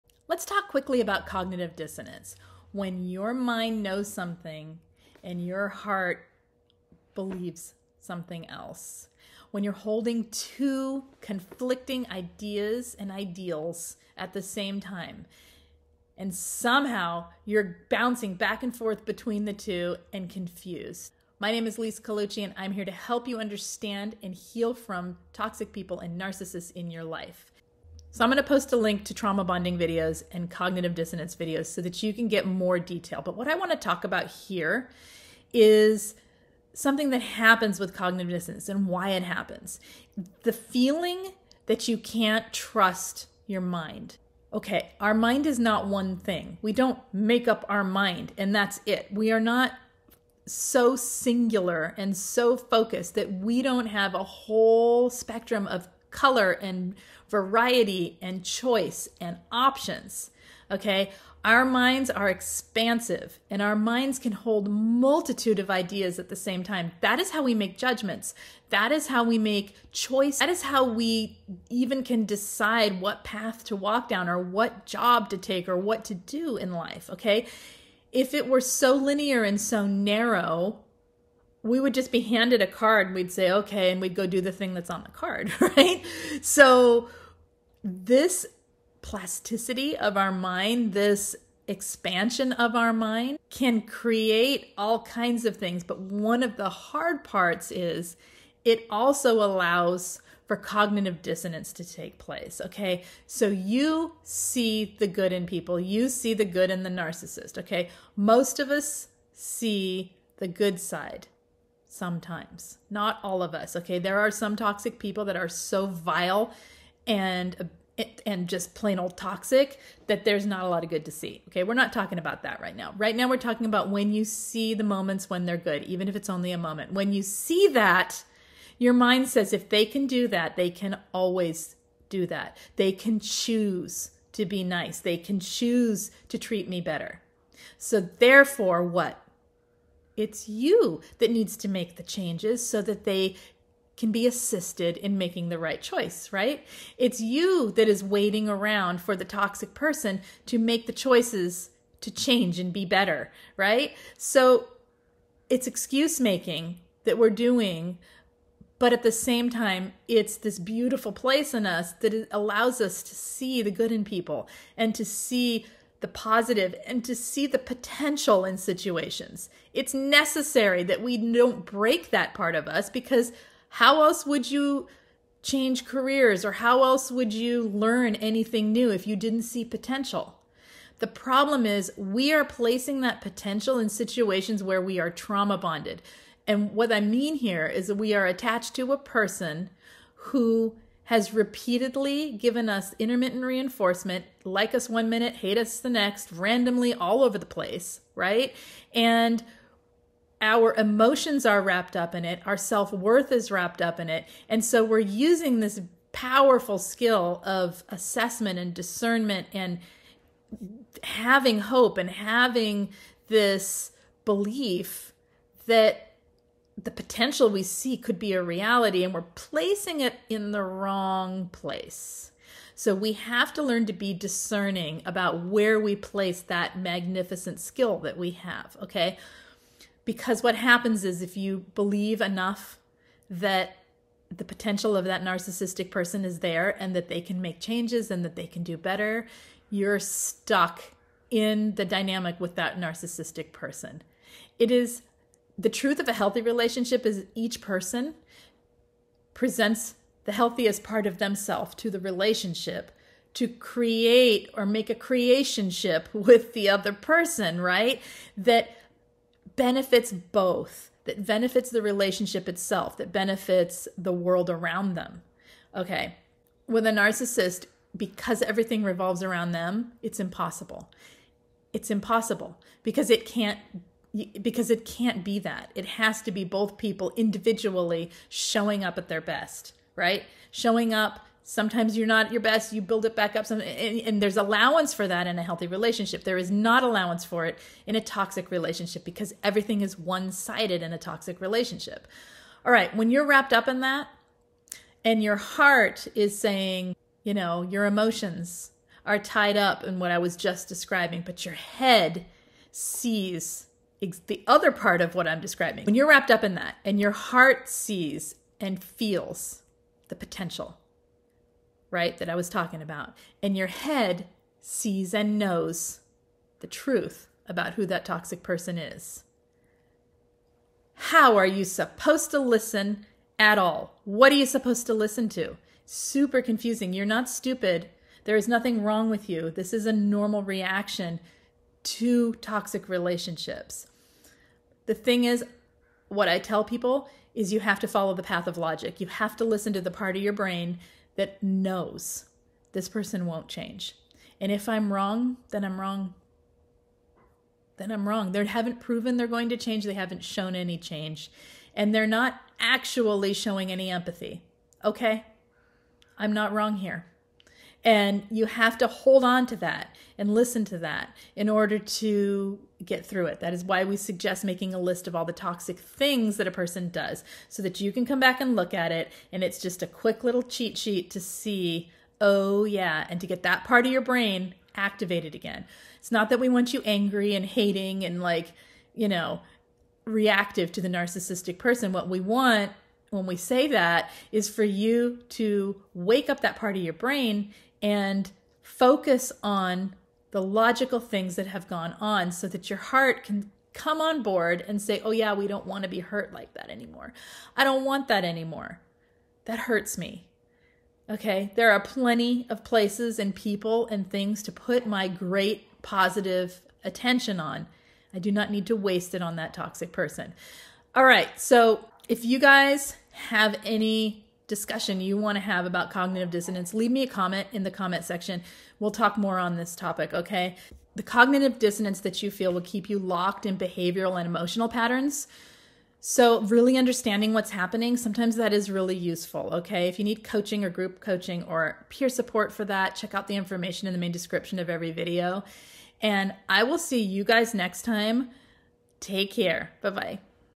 Do You Know They Are A Narcissist But Still Think They Might Change? Cognitive Dissonance
Do You Know They Are A Narcissist But Still Think They Might Change?   Cognitive Dissonance  Survivors of narcissistic abuse often grapple with cognitive dissonance. This arises when individuals encounter conflicting thoughts and emotions as the narcissist's actions contradict their initial charm. Survivors may find it challenging to reconcile the narcissist's initial appeal with later manipulative behavior, leading to self-doubt and confusion. Cognitive dissonance can impede survivors' ability to break free from the abusive relationship as they strive to make sense of the narcissist's behavior while preserving their own self-esteem.